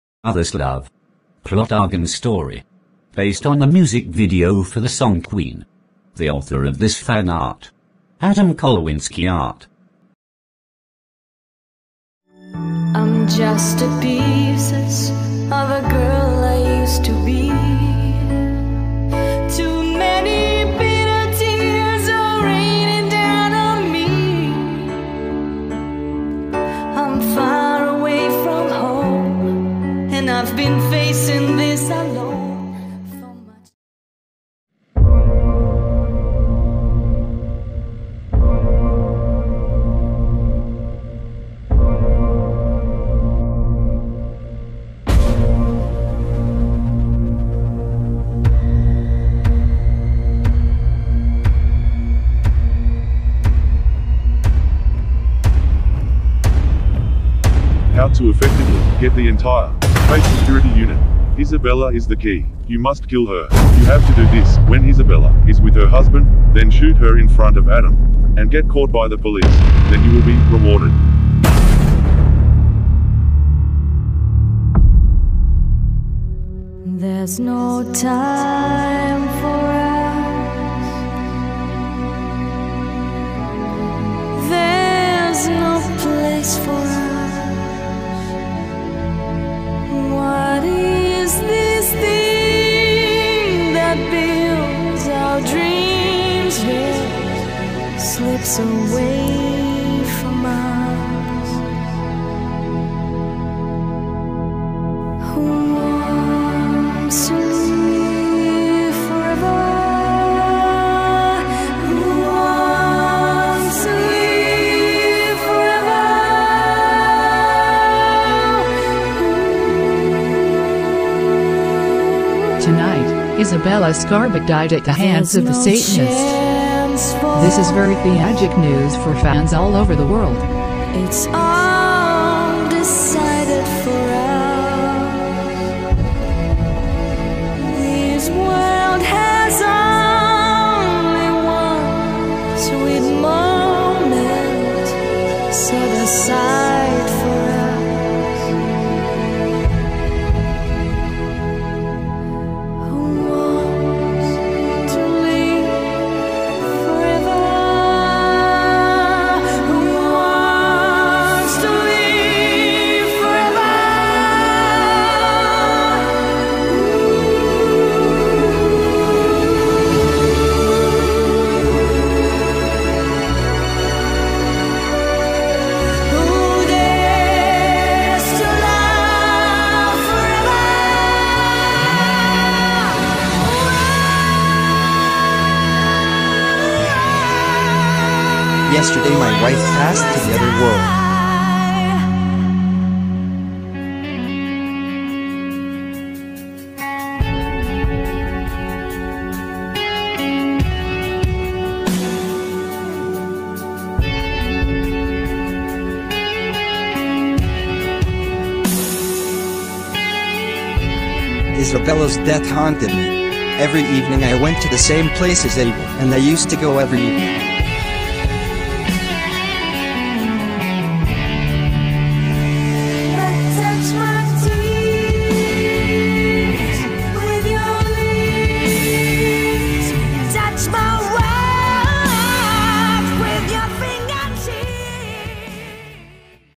Mother love Sezon II TRAILER / kryminal - sci fi
Agatha Christie, lady Mallowan, DBE, właśc. Agatha Mary Clarissa Miller Christie (ur. 15 września 1890 w Torquay, zm. 12 stycznia 1976 w Wallingford) – brytyjska autorka powieści kryminalnych i obyczajowych.

w drugim sezonie inpiruje sie kryminalami Agatha Christie,